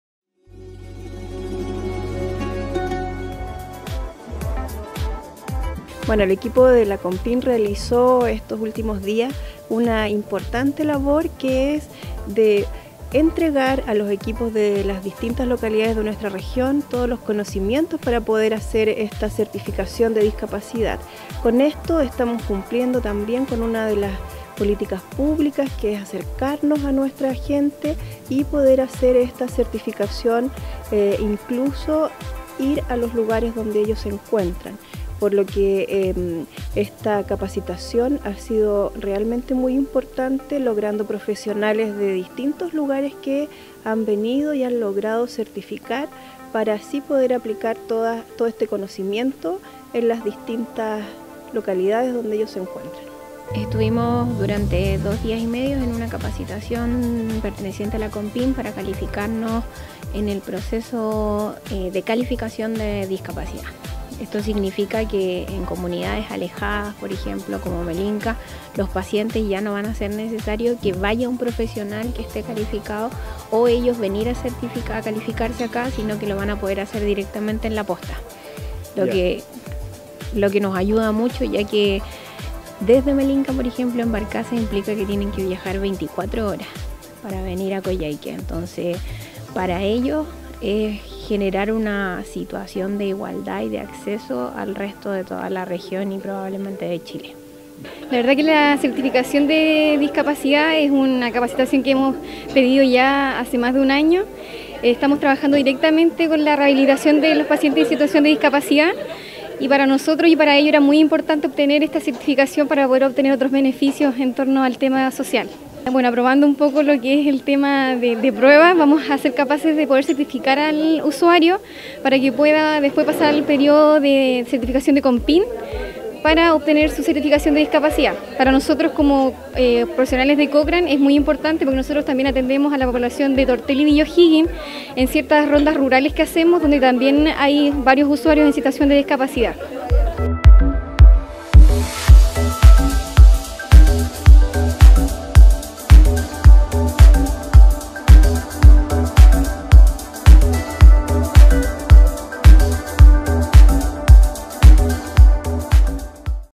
Capacitacion COMPIN
Profesionales de Salud se capacitan para certificar discapacidad en distintas comunas de Aysén